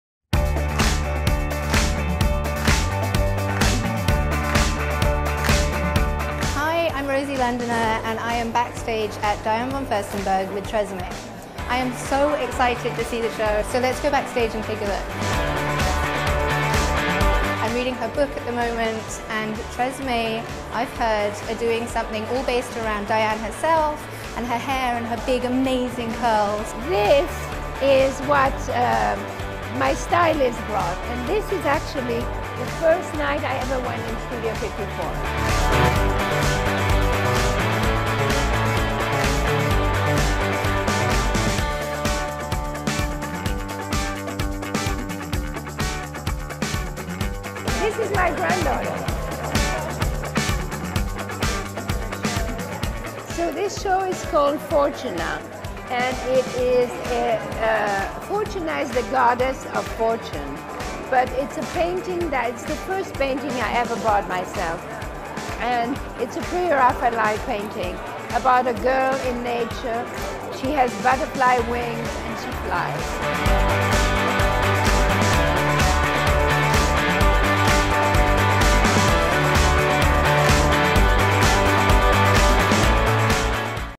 TRESemmé x Diane von Furstenberg En Locación con The Londoner
Entrá al backstage de NewYorkFashionWeek con TheLondoner y mirá el look de pelo de pasarela de @DVF’s .Conocé los productos de TRESemmé acá